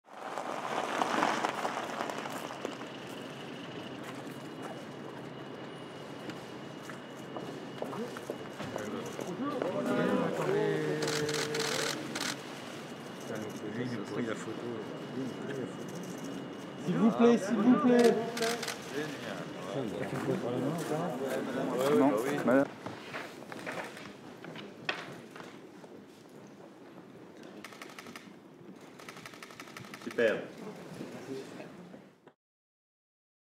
French President Emmanuel Macron holds talks with IMF chief Kristalina Georgieva | AFP
French President Emmanuel Macron holds talks with newly appointed International Monetary Fund managing director Kristalina Georgieva at the Elysee Palace. IMAGES